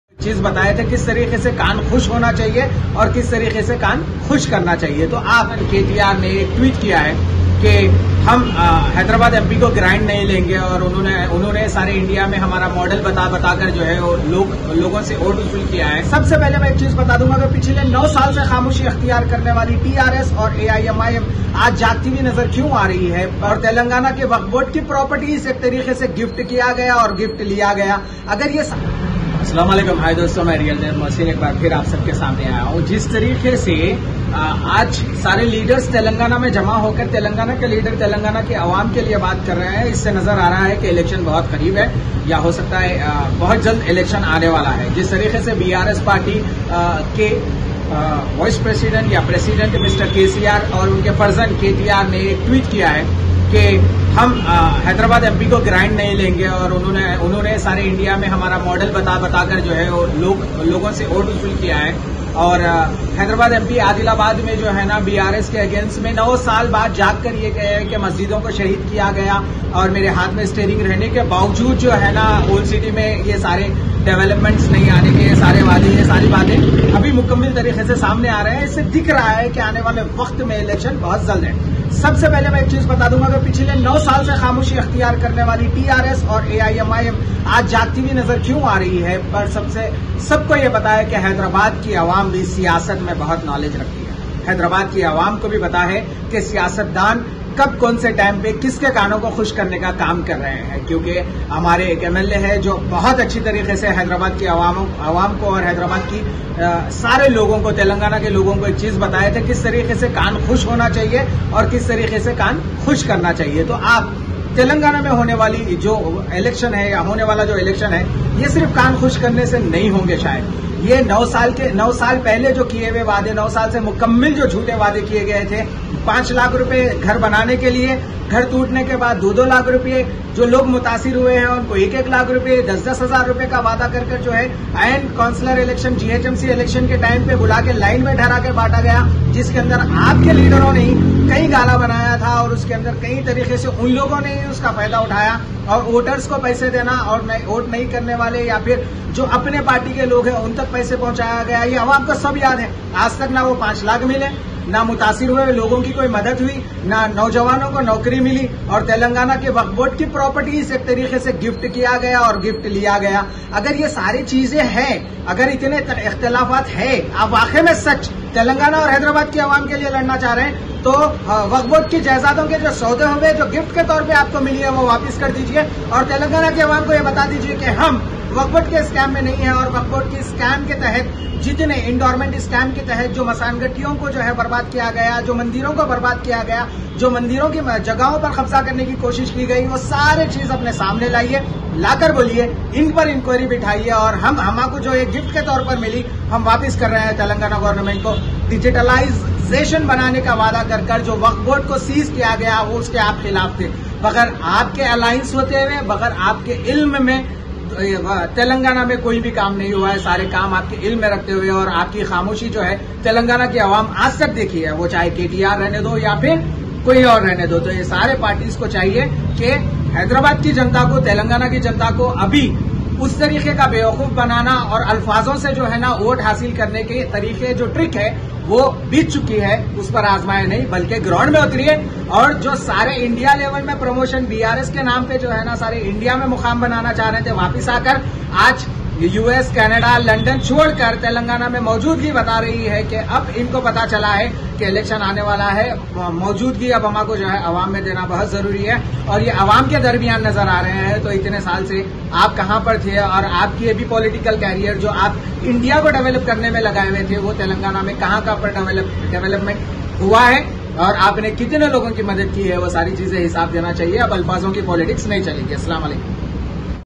Zabardast Tarike Se BRS Aur AIMIM Ek Dosre Se Khilaf Kya Yeh Sahi Hai ? Telangana mai .
Telangana Mein Election Ke Tayari Bade Zhooro Kharooz Ke Sath Shuru Hou Chuki Hai , Zabardast Tarike Se BRS Aur AIMIM Ek Dosre Se Khilaf Kya Yeh Sahi Hai ? Aab Alfazon Ki Politics Nahi Chalenge.